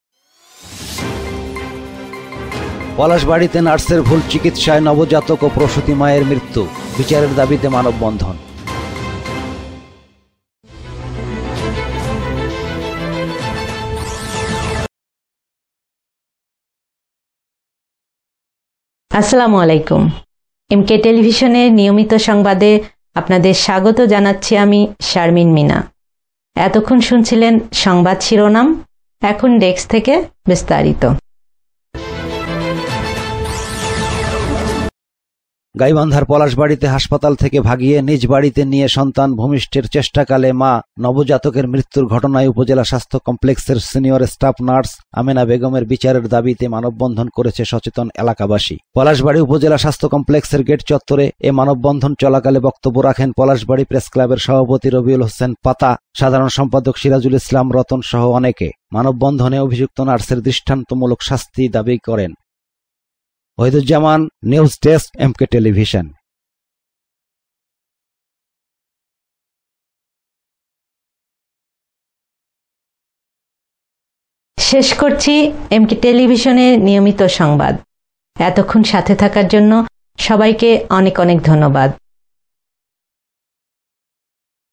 পলাশবাড়িতে নার্সের ভুল চিকিৎসায় নবজাতক ও প্রসূতি মায়ের মৃত্যু। MK TV BD
পলাশবাড়িতে নার্সের ভুল চিকিৎসায়  নবজাতক ও প্রসূতি মায়ের মৃত্যু